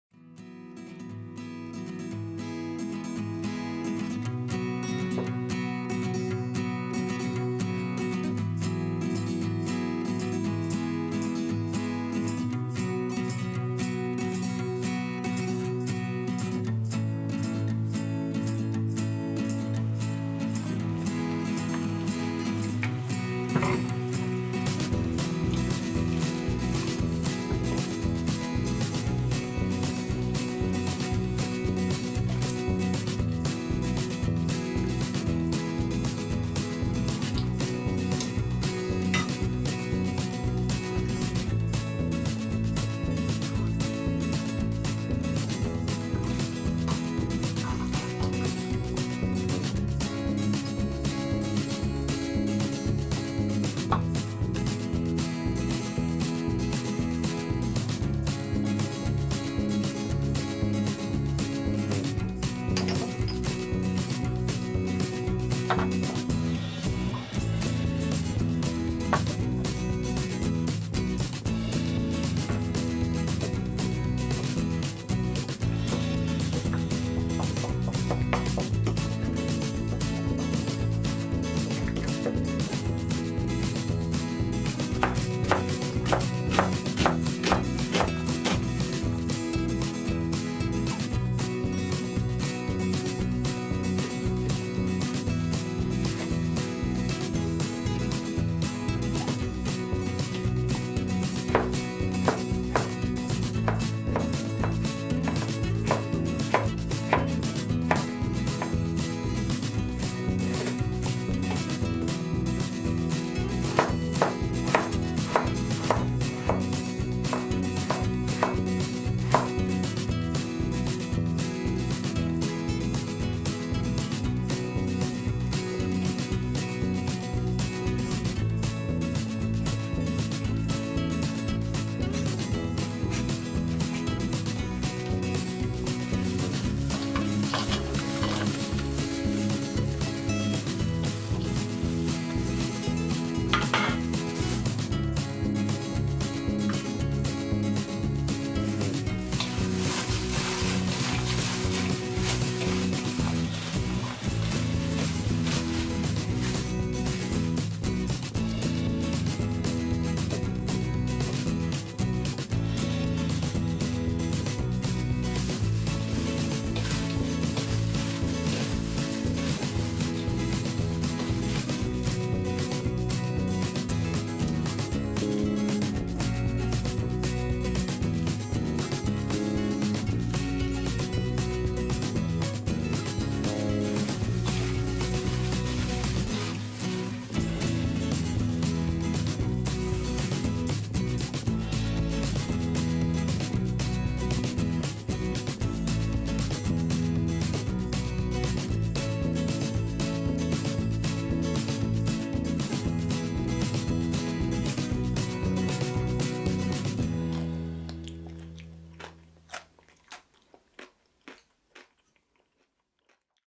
오징어초무침, 새콤달콤 입맛돋는 미나리오징어초무침
새콤달콤 양념으로 맛있게 무쳐내는 오징어초무침!
제철 미나리까지 넣어 맛있게 만들어드세요 :)